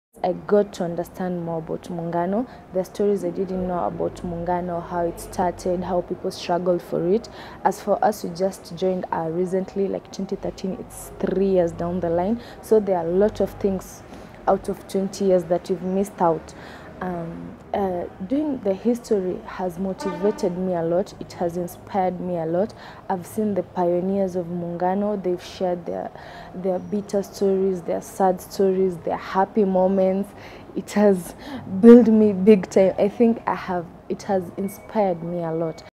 Sophia Khamis 2/4: What has it been like documenting the history of Muungano?
This interview is with:

Sophia Khamis
Muungano & History documentation team
From: Ugeni, Machakos
Interview: 6 May 2016, Nairobi
Interviewer(s): Kate Lines
Original language: English